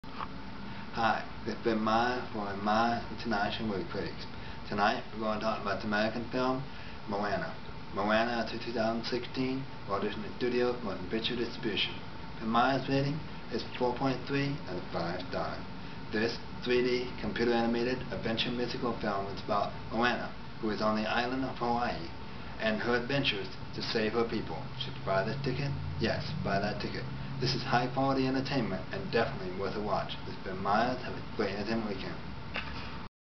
Moana (2016)
Ben Meyers International Movie Critics offers a written review of “Moana” (2016)